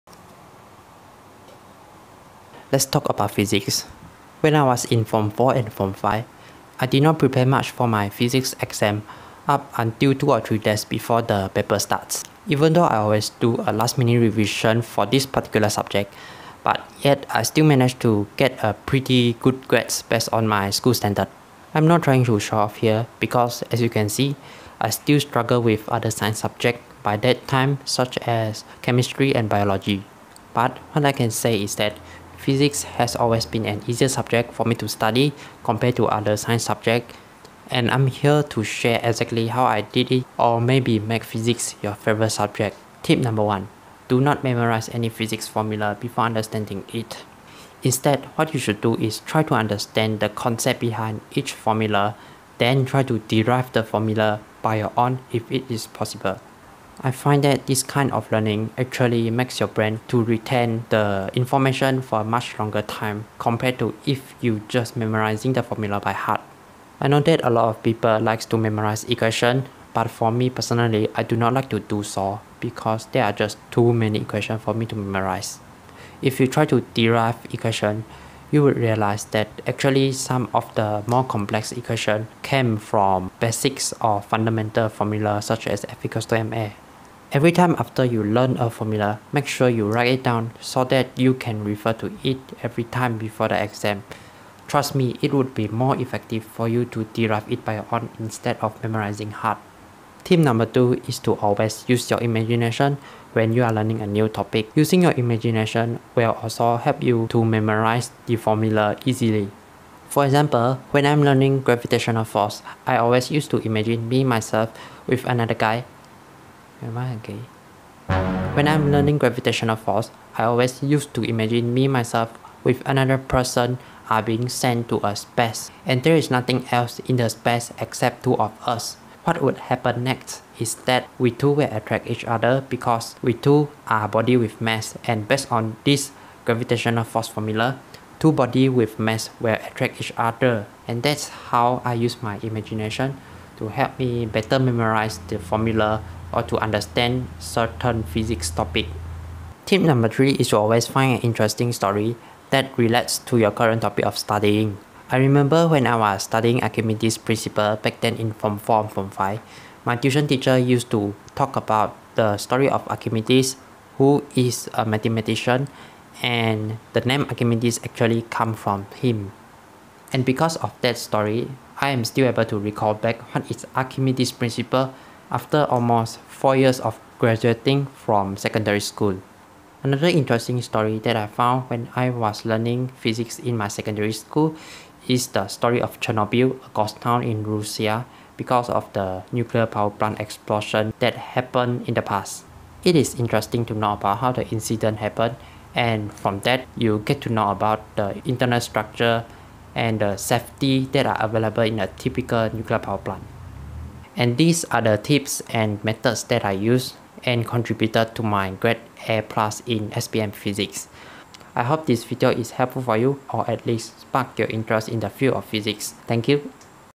Score A+ in SPM physics easily || Secrets that top student didn’t tell you
During my SPM time , Physics was relatively easier for me to study compared to other Science subject , In this short video, I share about some of the important study techniques/tips that I always use when I study Physics.

0:00  Introduction
0:43 Tip 1: Never memorize formula without understand it
1:40 Tip 2:  Always use your imagination
2:29 Tip 3: Find interesting stories that are related to Physics